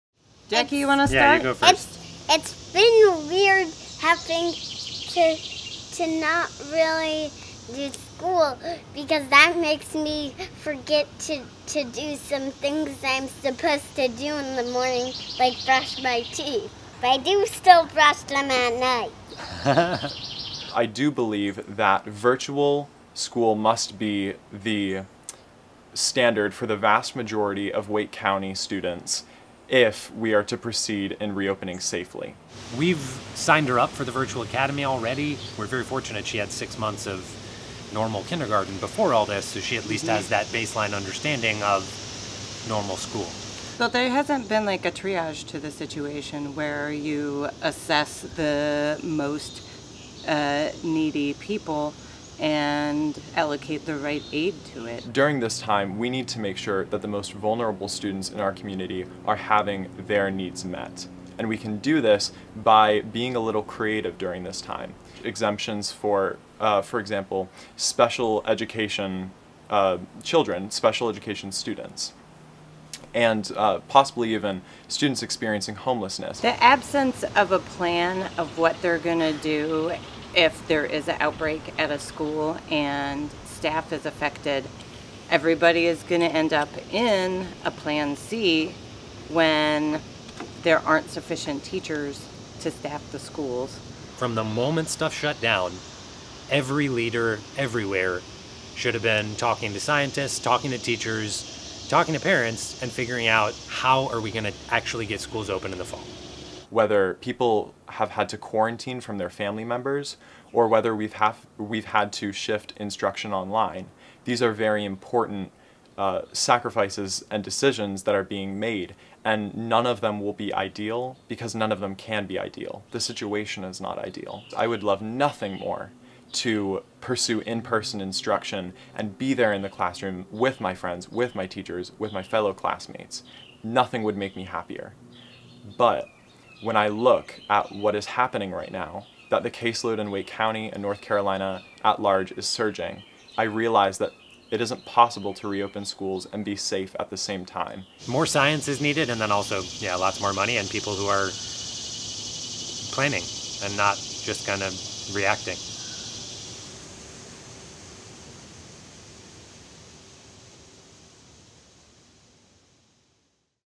Concerned students and parents in Wake County choose Virtual Academy
Jason and Kate Siegel have enrolled their rising first grader, Jacqueline, 6, in Wake County's Virtual Academy. Brien Brennan, a rising senior, started a petition for the full implementation of virtual learning for WCPSS schools this fall.
Video by Julia Wall / The News & Observer